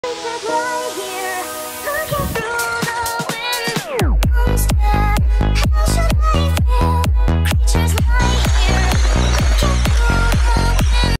Stunt Riding Toddler | Mini Electric Motorcycle | Stacyc E-Bike
Dirt Jumps, Donuts and Burnouts at Stevie Smith Memorial Bike Park.

12” ion e-drive Harley davidson edition stacyc bike.

* 3 years 9 months old *